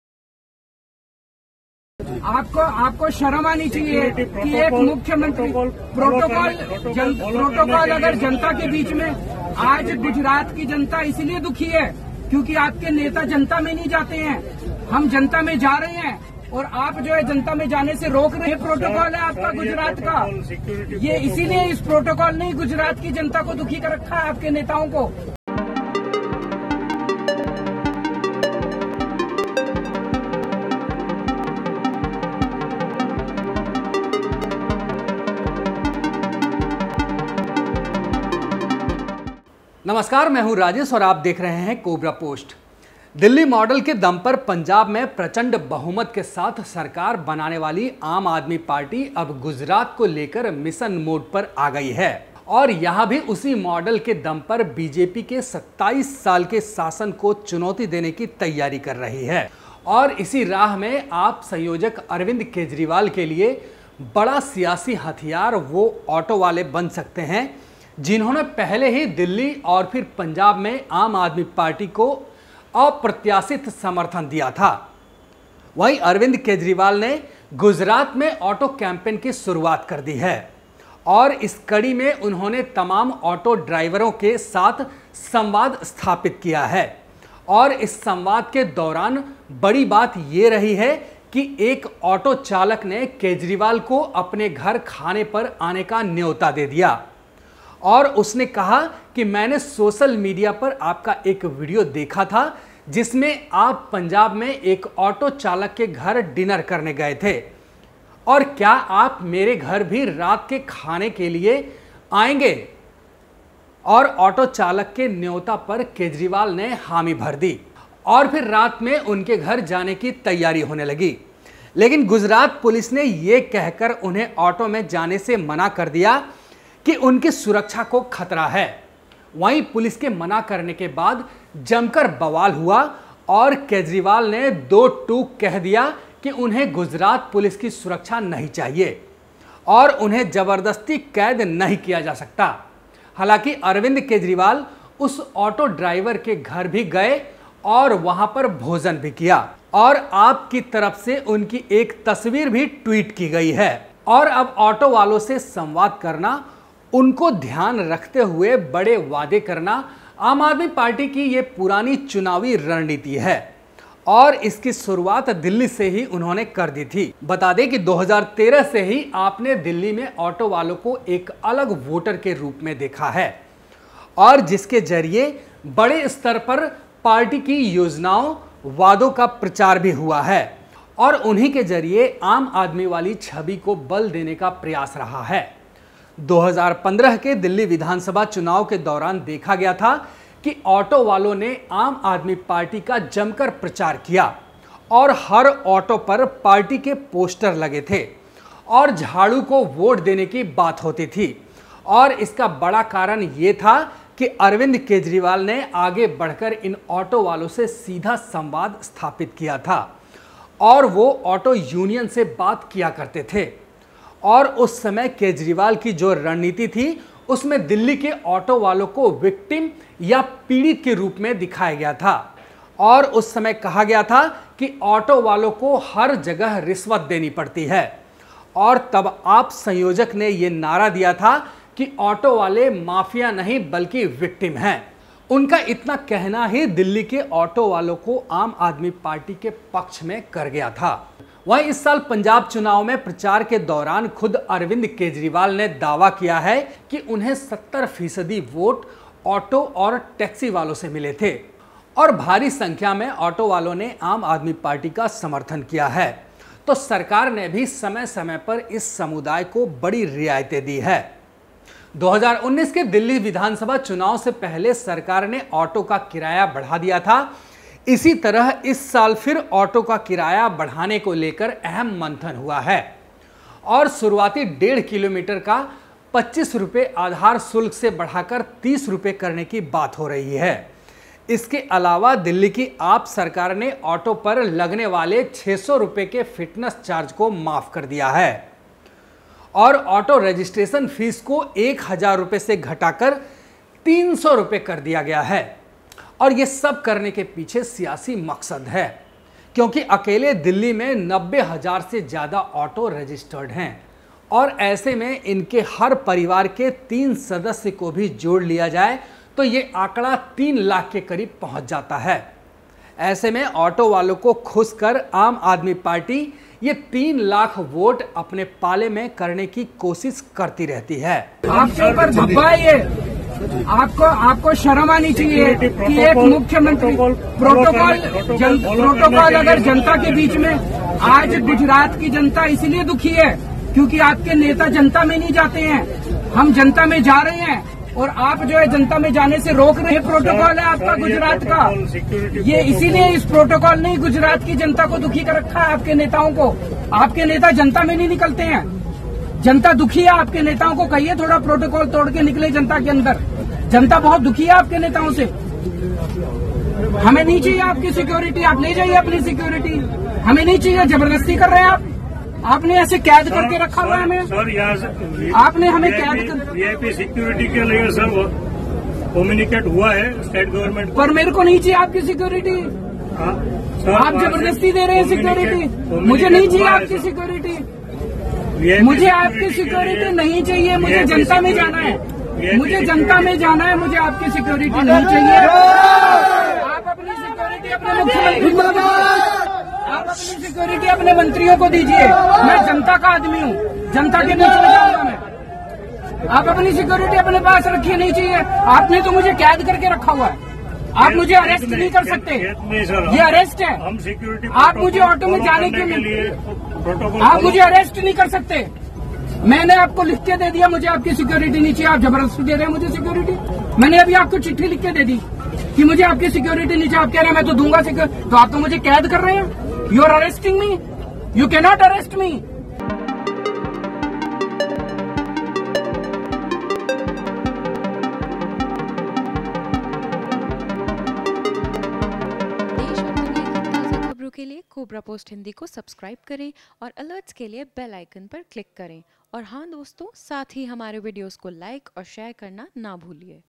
अरविंद केजरीवाल को ऑटो में बैठने से गुजरात पुलिस ने रोका, बीच सड़क पर हुआ हंगामा| Cobrapost Hindi
@Cobrapost Hindi
@coprapost अरविंद केजरीवाल को ऑटो में बैठने से गुजरात पुलिस ने रोका, बीच सड़क पर जमकर हुआ हंगामा